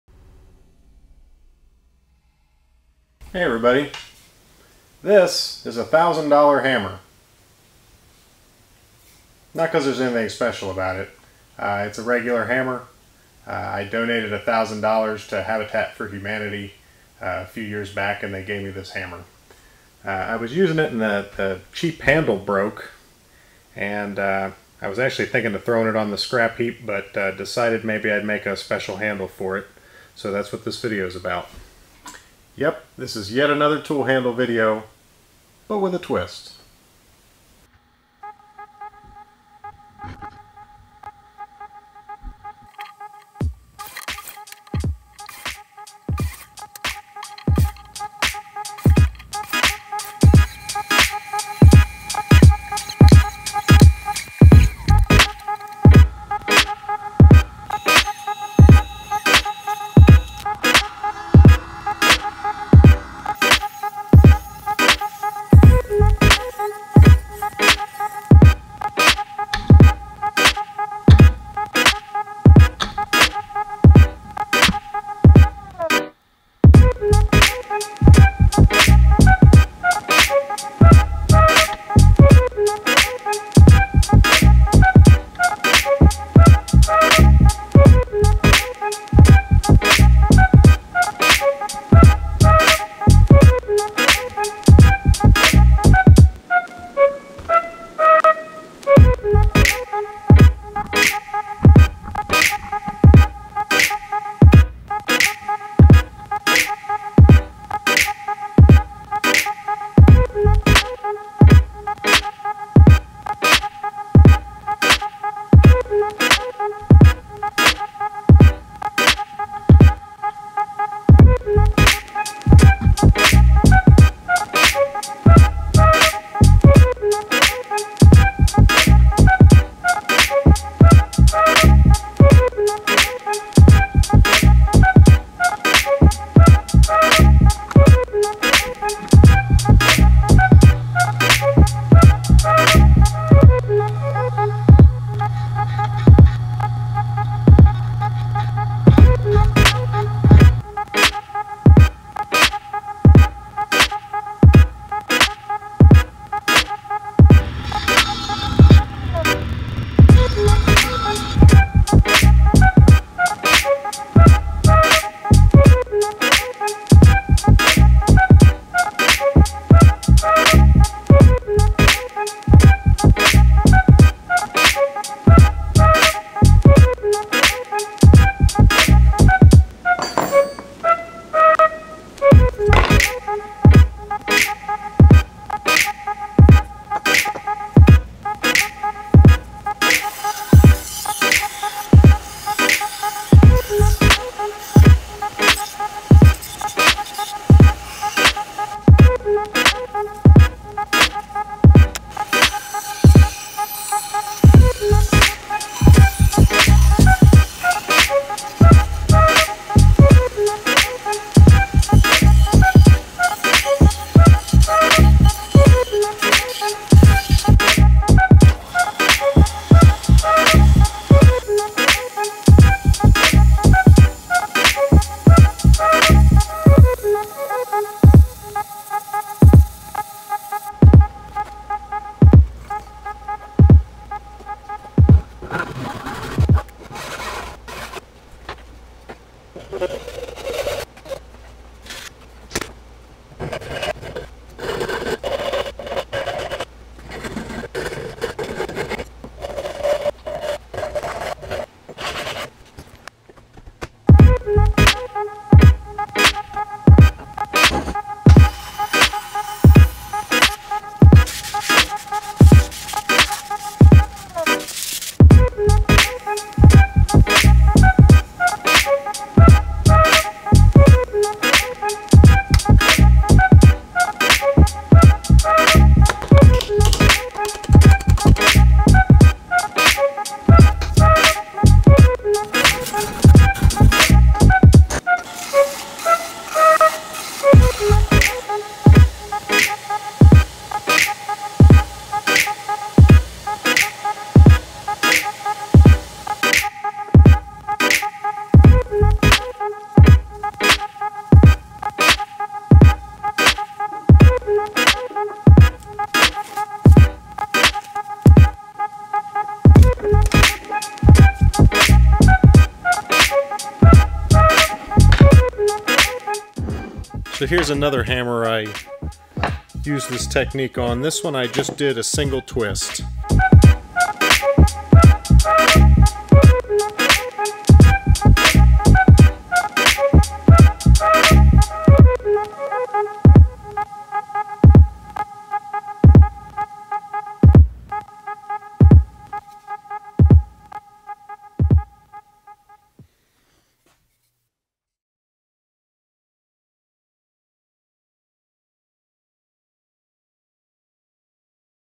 Twisted laminate hammer handles. Strong and Unique! FarmCraft101
Putting a new twist on an old idea: the tool handle.  I've been wanting to try this for awhile, and when the handle broke on this hammer I decided it was time.  It was an experiment, and turned out nicely in my opinion.

I made my twist blanks 1.5" square.  That works fine if you want to do a single twist handle, as shown toward the end of the video.  In the video, I made two twist blanks, twisting them in opposite directions, and then cut them in order to make a book-matched blank.  My waste pieces from these cuts were pretty thin, and could possibly be used for a very small handle, but really aren't much use.  If I were to do it again, I would make the blanks 2" square, and then you could definitely get 2 nice handles out of it.

With modern glues, the laminated construction of these handles definitely makes them stronger than solid wood, and much less likely to split.  I don't think the twist adds any strength, and is just for looks.  Some might argue that the twist could detract from the strength of the handle, but I doubt it.  Time will tell.

I have been putting a lot of work into these vids!